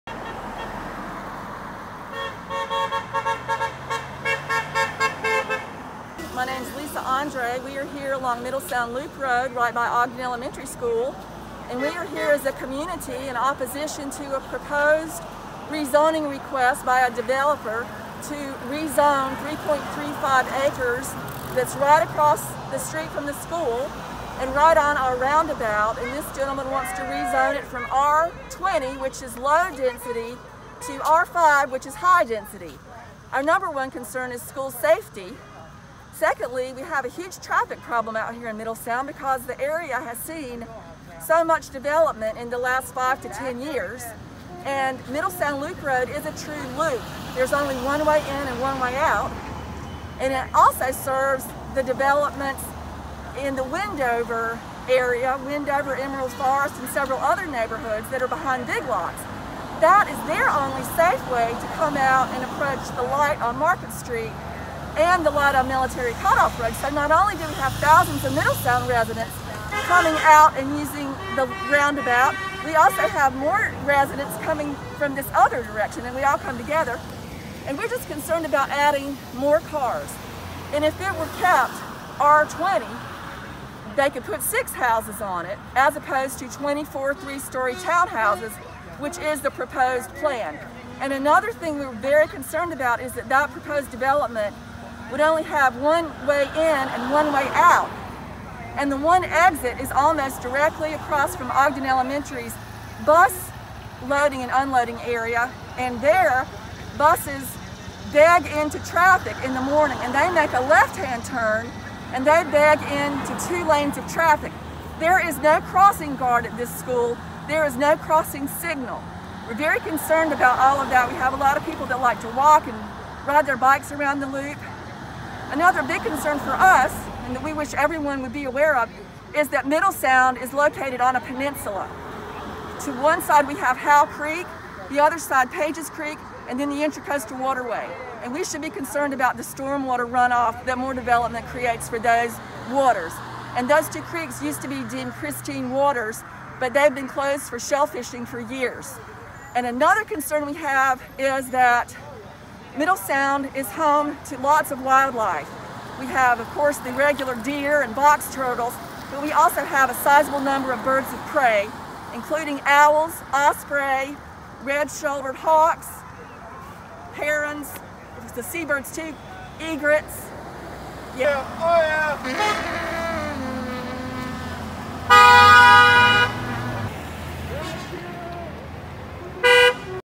Ogden Protest Against Proposed Development
Residents of the Middle Sound neighborhood protested on Saturday morning against the proposed development of twenty-four, three-story townhouses that would be located across from Ogden Elementary School.